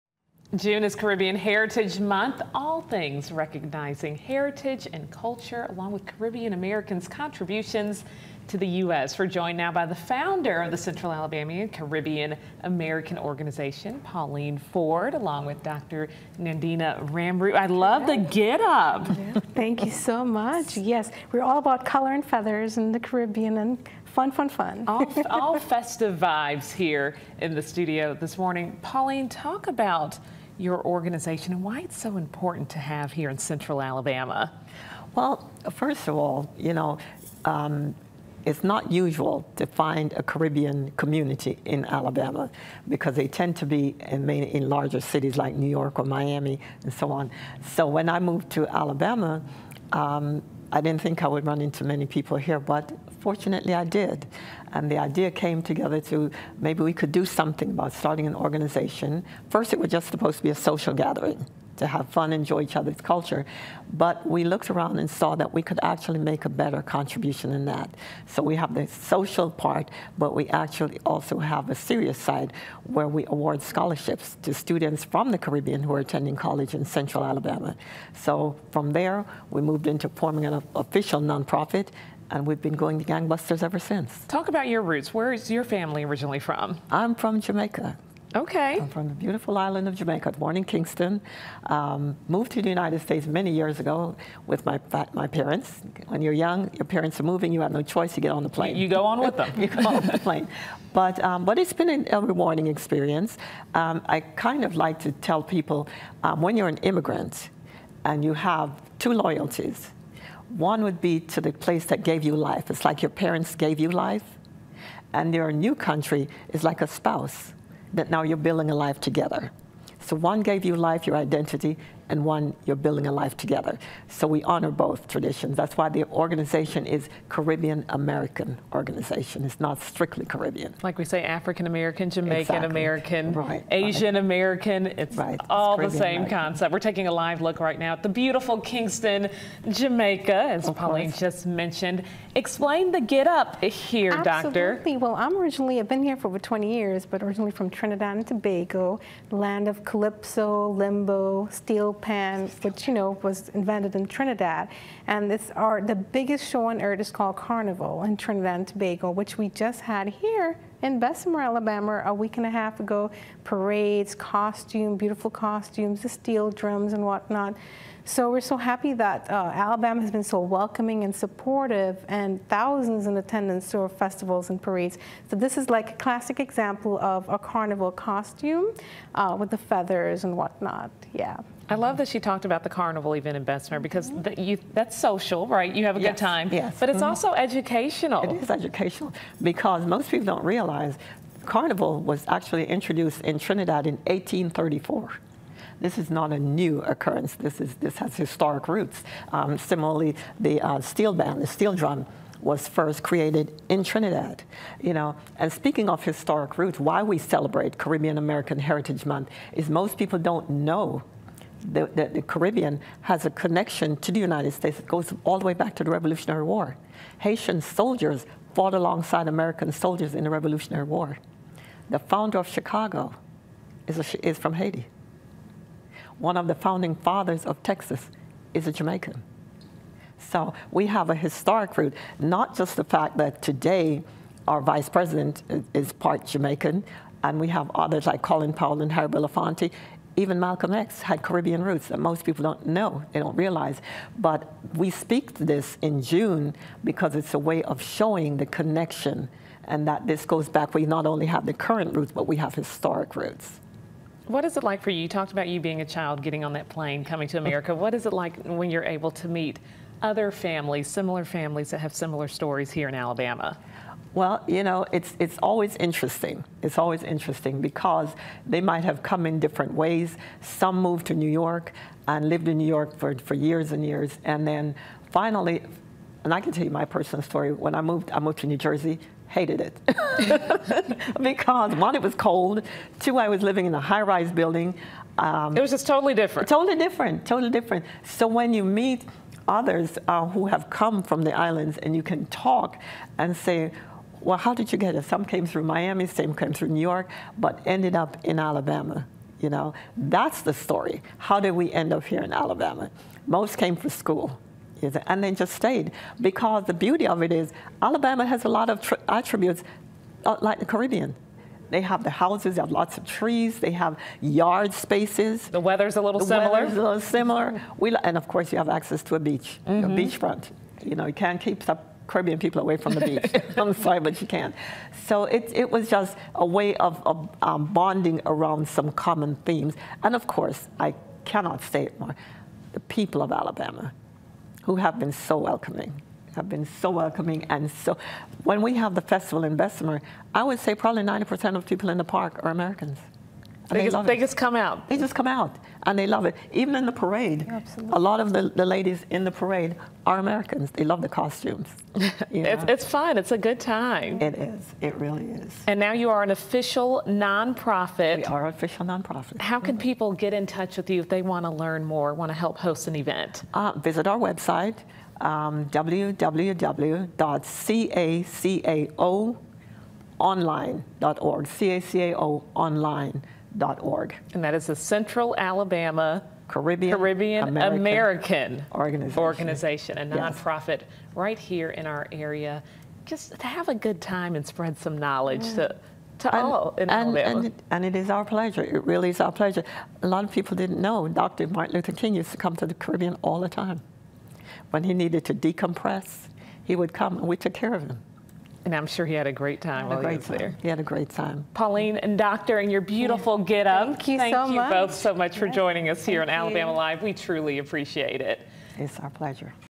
June is Caribbean Heritage Month
Caribbean Heritage Month is a time to recognize the culture and heritage of Caribbean Americans and their contributions to the U.S.